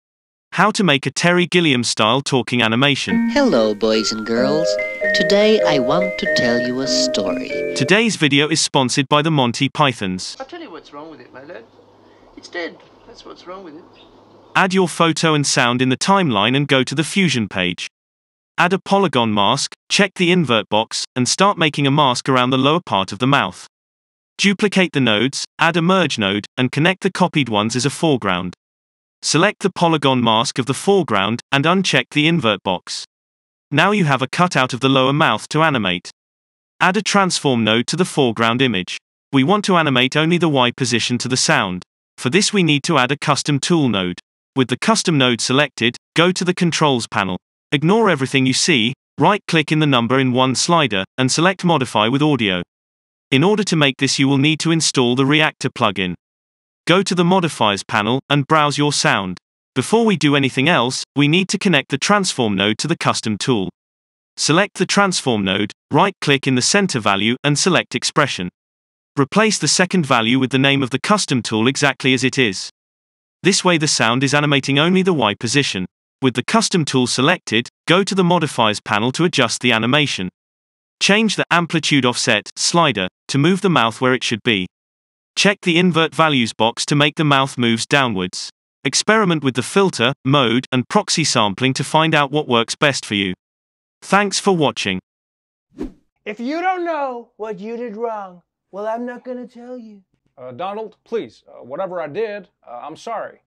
How To Make A Talking Animation | DaVinci Resolve 17
How to make a talking animation like Monty pythons,
for this, you will need to download and instal the reactor plug-in, and install the suck less audio modifier.

If you are thinking to buy a new camera you can try one of those great cameras :
Sony Alpha 7S III
Blackmagic pocket 6K
sony a6500